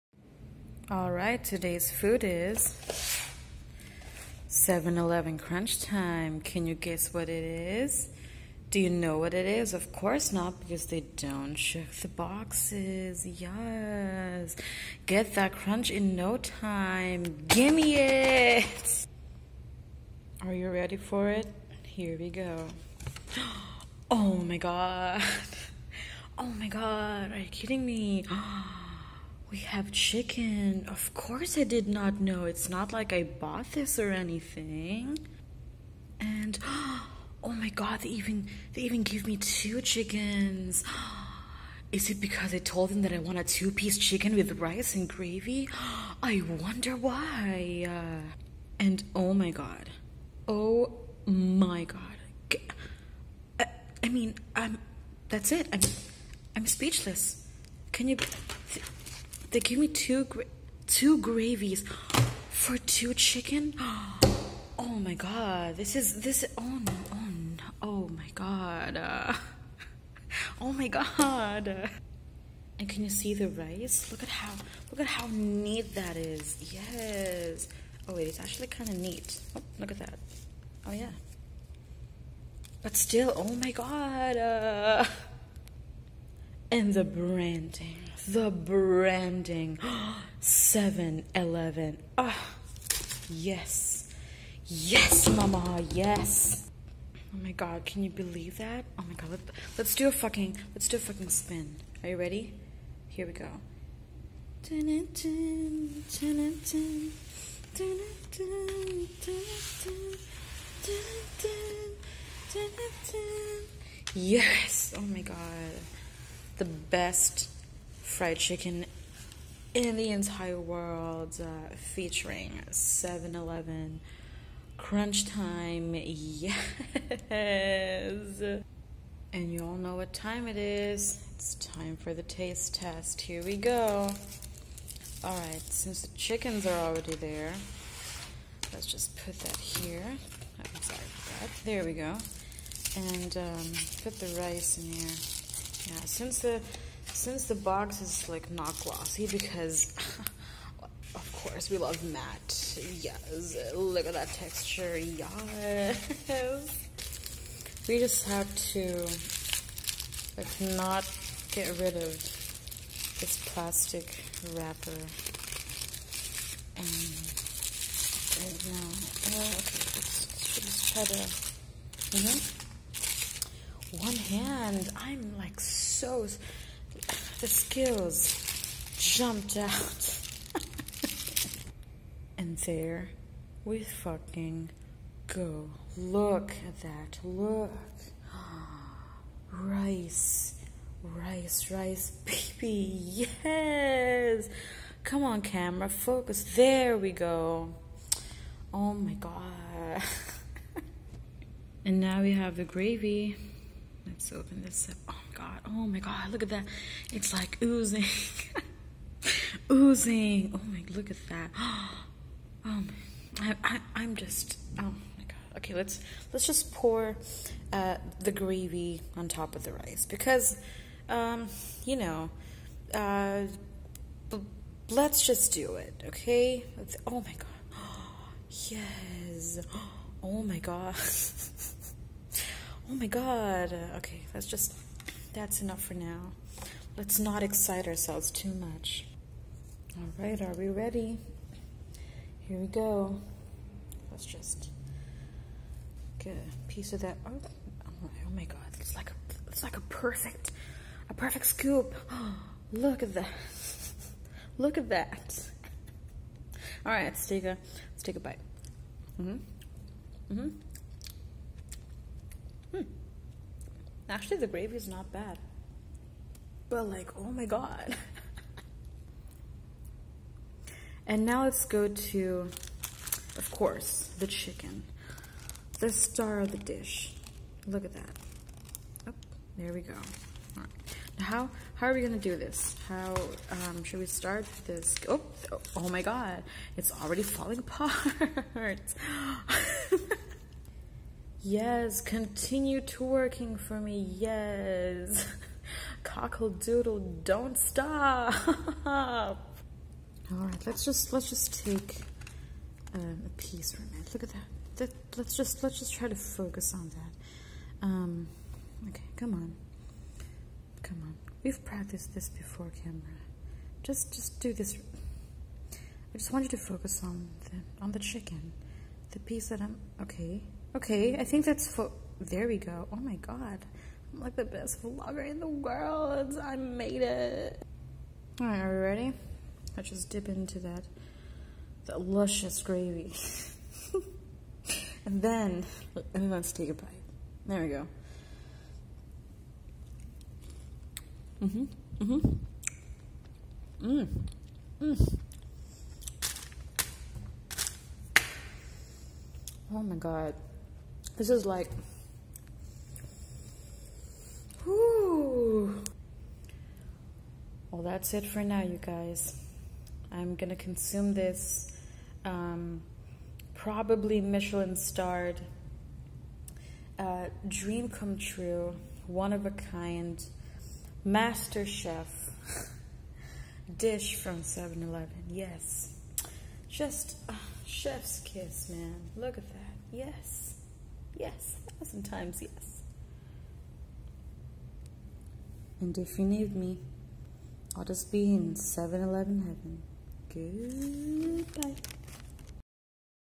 Eating 7-Eleven Crunch Time Fried Chicken | laudclaud
another round of convenience store meals!

listen- fried chicken is LIFE. watch me convince myself that this fried chicken is top-tier 😂

1 LIKE = 1 cock-a-doodle doo